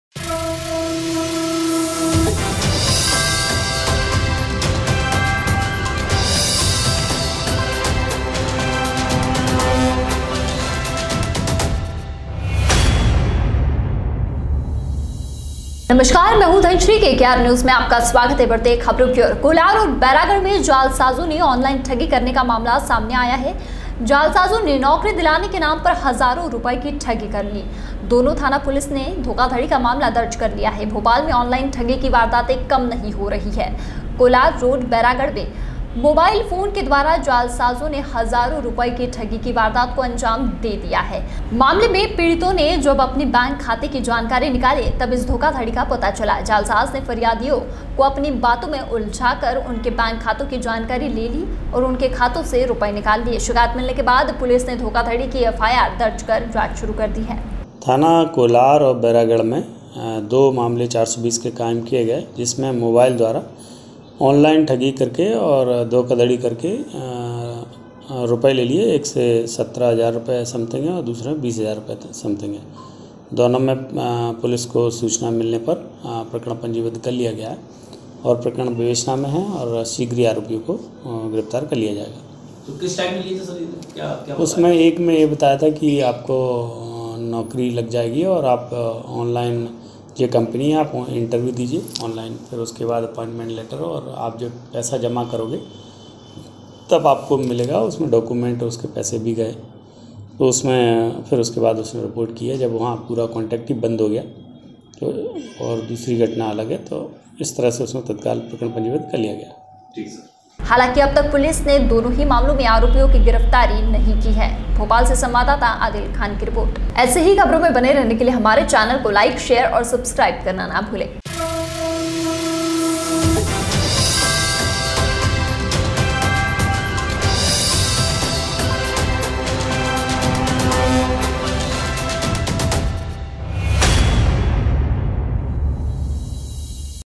नोकरी दिलवाने के नाम पर ऑनलाइन ठगी मोबाइल फ़ोन के माध्यम से की धोखाधड़ी।
एंकर -कोलार ओर बैरागढ़ में  जालसाजो ने ऑनलाइन ठगी करने का मामला सामने आया है।जालसाजों ने नोकरी दीलवाने के नाम पर हज़ारों रुपये की ठगी कर ली।दोनो थाना पुलिस ने धोखाधड़ी का मामला दर्ज कर लिया है।

 वीओ -भोपाल में ऑनलाइन ठगी की वारदातें कम नही हो रही है।कोलार ओर बैरागढ़ में मोबाइल फोन के द्वारा जालसाजों ने हजारों रुपये की ठगी की वारदात को अंजाम दे दिया। मामले में पीड़ितो  ने जब अपने बैंक खाता की जानकारी निकाली, तब इस धोखाधड़ी का पता चला।जालसाज ने फरियादियो  को अपनी बातों में उलझाकर उसके बैंक खाते की जानकारी ले ली ओर उनके खातों से रुपये निकाल लिए।
शिकायत मिलने के बाद पुलिस ने धोखाधड़ी की एफआइआर दर्ज कर जांच शुरू कर दी है।

बाईट-दिनेश कुमार कौशल, एडिशनल डीसीपी

हालांकि अब तक पुलिस ने दोनो ही मामलों में आरोपीयो की गिरफ्तारी नही की है।

 भोपाल से संवाददाता आदिल खाँन कि रिपोर्ट।